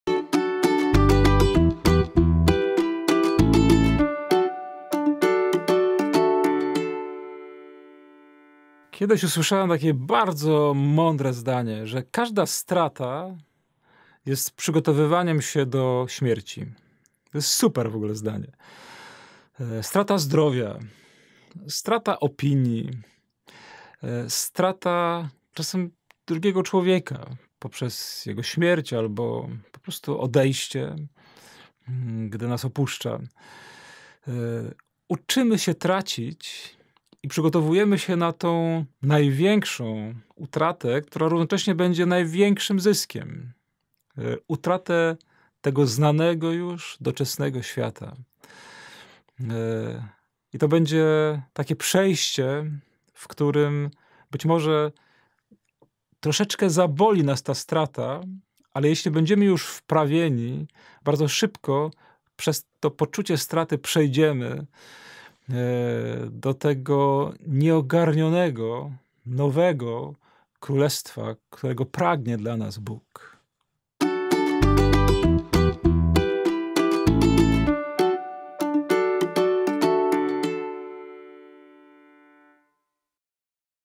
EwangeliarzOP ll 15 listopada 2019 ll Łk 17, 26-37
Codzienny komentarz do Ewangelii  o. Wojciecha Jędrzejewskiego OP.

Jezus powiedział do swoich uczniów:

«Jak działo się za dni Noego, tak będzie również za dni Syna Człowieczego: jedli i pili, żenili się i za mąż wychodziły aż do dnia, kiedy Noe wszedł do arki; a przyszedł potop i wygubił wszystkich.

Podobnie jak działo się za czasów Lota: jedli i pili, kupowali i sprzedawali, sadzili i budowali, lecz w dniu, kiedy Lot wyszedł z Sodomy, spadł z nieba „deszcz ognia i siarki” i wygubił wszystkich; tak samo będzie w dniu, kiedy Syn Człowieczy się objawi.

Powiadam wam: Tej nocy dwóch będzie na jednym posłaniu: jeden będzie wzięty, a drugi zostawiony. Dwie będą razem mleć na żarnach: jedna będzie wzięta, a druga zostawiona».

Pytali Go: «Gdzie, Panie?» On im odpowiedział: «Gdzie jest padlina, tam zgromadzą się i sępy».